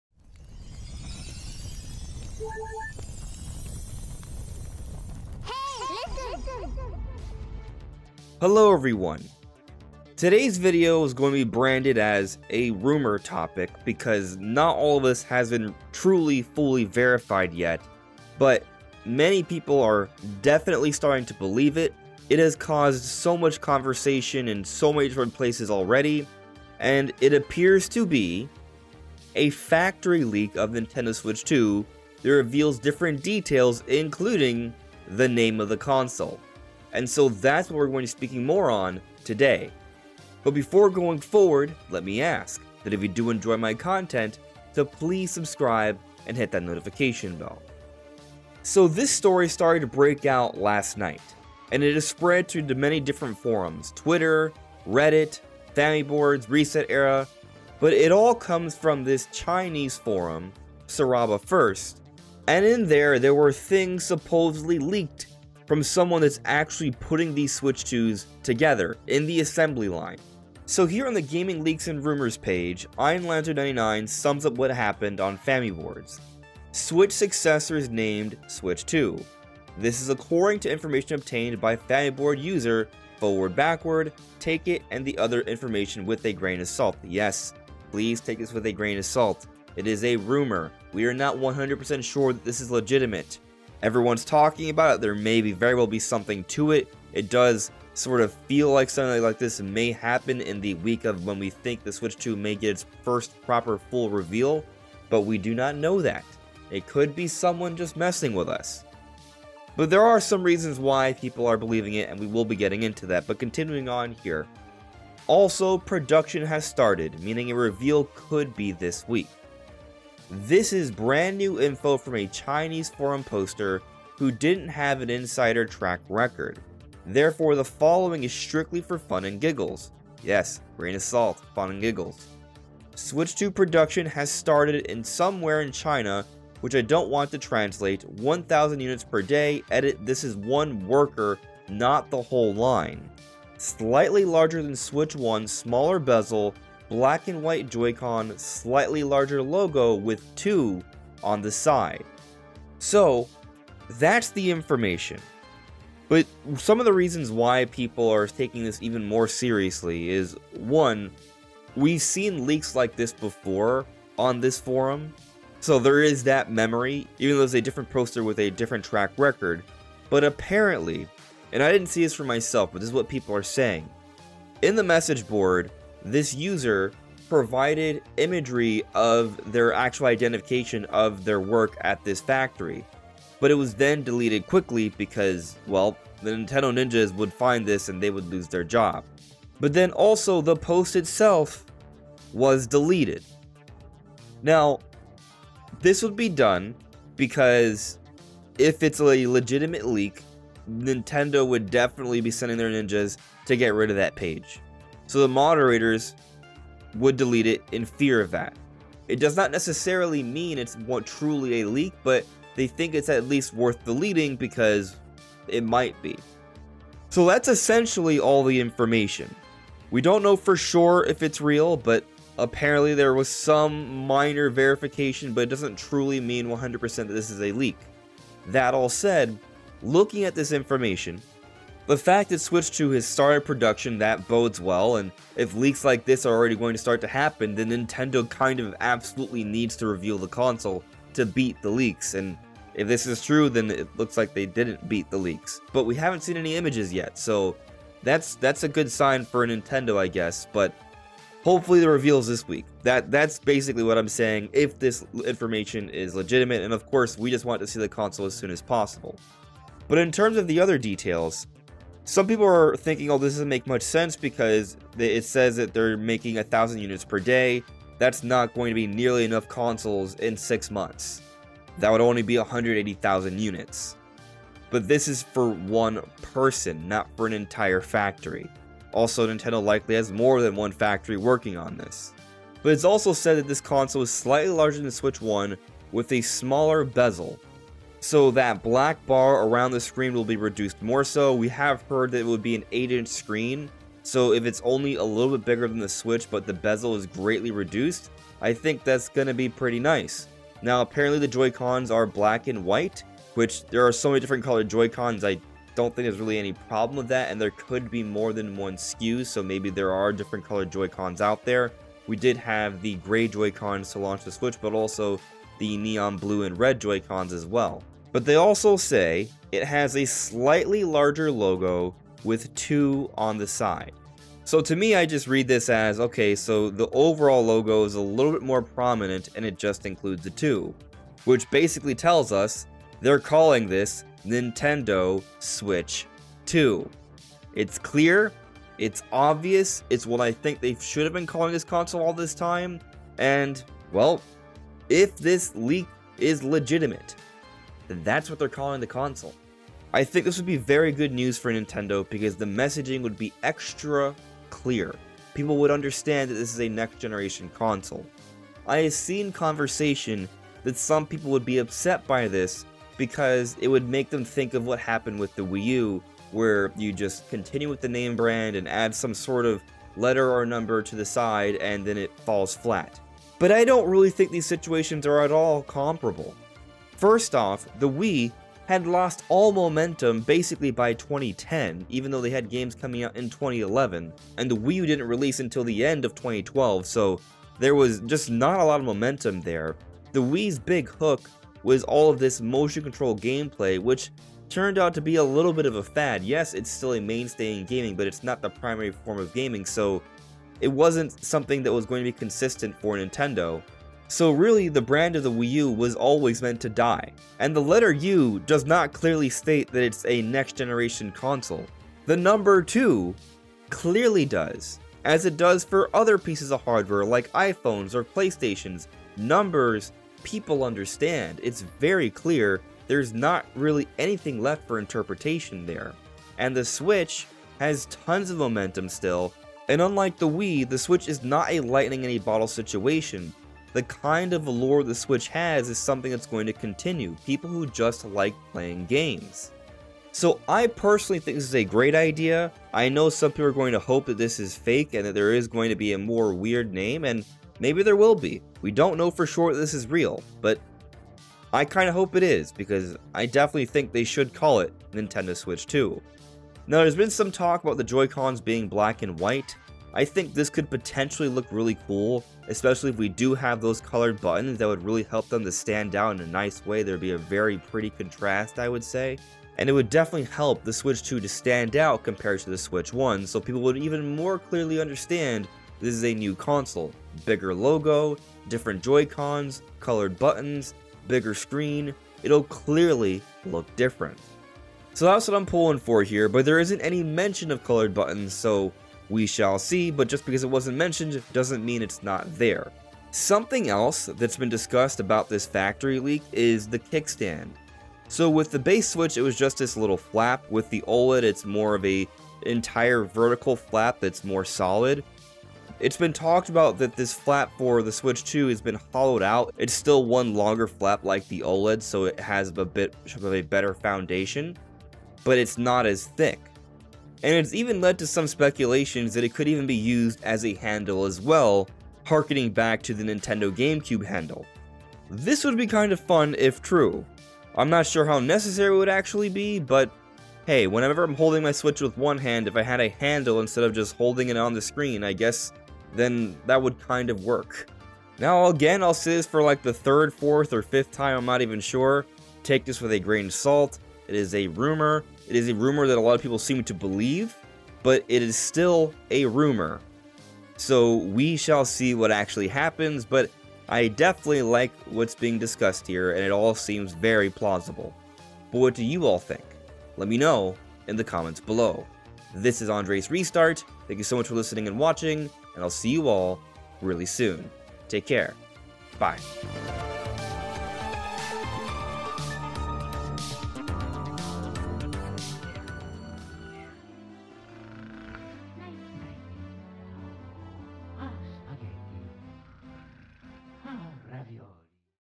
Nintendo Switch 2 Factory Leak Reveals Name And More???
The Nintendo Switch 2 name and other details may have been reveal by this latest possible factory leak. Is this rumor legitimate? Is a reveal soon? Let's talk about it.